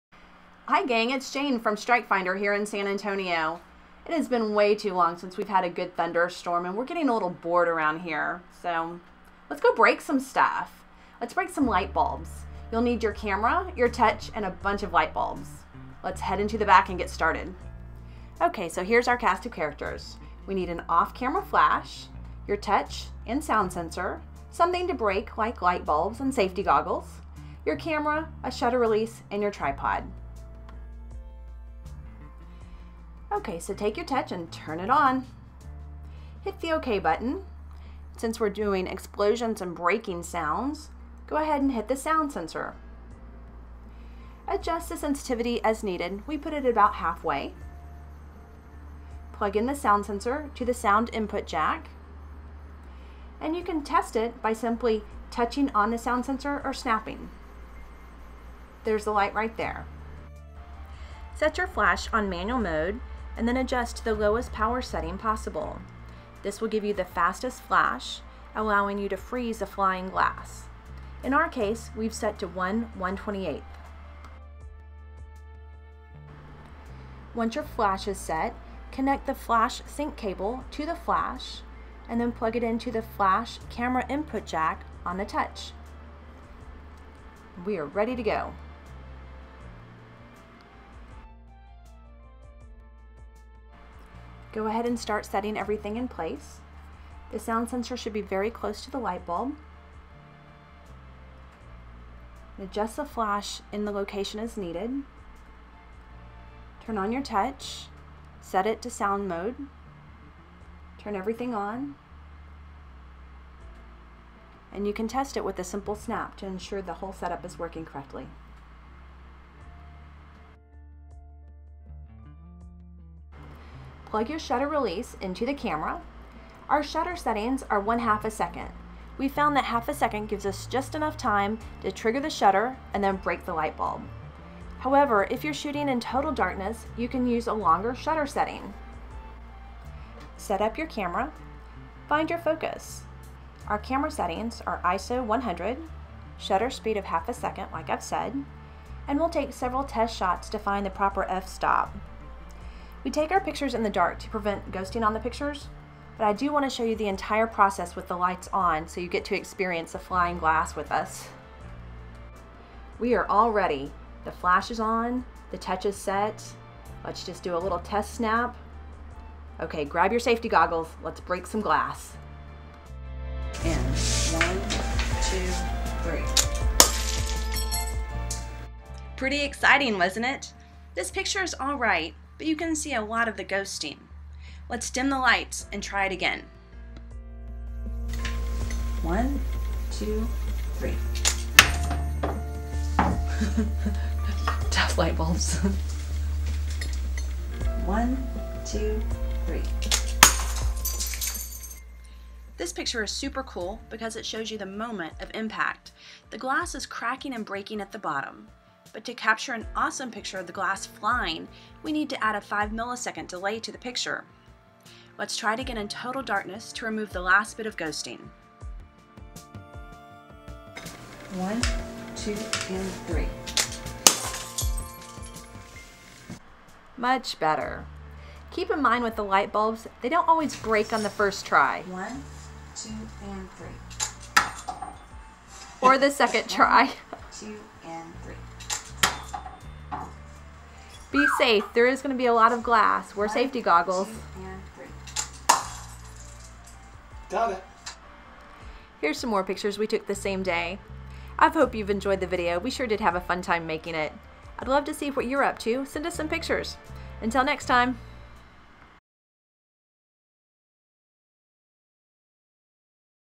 How to Capture Explosions using Sound Sensor
How to Capture Explosions

It’s been too long since we’ve had a good thunderstorm! Let’s combat boredom by breaking some light bulbs.

You’ll need an off camera flash, your Touch and sound sensor, plenty of light bulbs to break, safety goggles, camera, shutter release and your tripod.

Set up your Touch
After turning on your Touch, hit OK, and select Sound since we’re doing explosions.  Adjust sensitivity as needed. If the sound is soft or distant, increase the sensitivity. If it’s a loud sound you can leave the sensitivity low. We set ours to half way, which is really overkill for our example.

Plug the sound sensor into the Touch at the input jack labeled Sound. You can test the sound sensor by simply tapping on the sound sensor or snapping. The red light on the Touch will light up to indicate everything is plugged in correctly.

Off Camera Flash
Set your flash on manual then on the lowest setting which will provide the fastest flash allowing you to freeze the action. In our case, the flash setting is 1/128. Connect the flash to the Touch with the flash sync cable. This will allow the Touch to trigger the flash at the moment of impact.

Set the players in their places
The sound sensor should be very close to the source of the explosion. This will provide the shortest time possible for the Touch to trigger the flash. Place the flash in the best location to light up the flying glass or as you intend the shot to look.

Set up your Camera
We use a shutter release to fire the camera. The shutter is set to half a second. This allows us just enough time to open the shutter then break the light bulb. Since the shutter is set for such a long time, we shoot in the dark to eliminate the ghosting effect on the photos.

Camera Settings:
ISO 100
Shutter half a second
We found our F-stop by taking several test shots.

I want to show you the process in the light so you can experience all the flying glass with us. It’s pretty exciting!

The photo with the lights on clearly shows the ghosting. Let’s dim the lights and try again.

I think it’s hilarious that the light bulbs don’t always break! When they do break however, the glass will fly. Be sure to wear safety goggles.

The second photo is super cool because it shows the moment of impact, where the bulb begins to break at the bottom. However, to capture the full effect of flying glass we need to add a 5 ms delay to the Touch. Let’s try again in complete darkness, which will remove any remaining ghosting.

What are you capturing these days? Please share your work with us, we’d love to see it!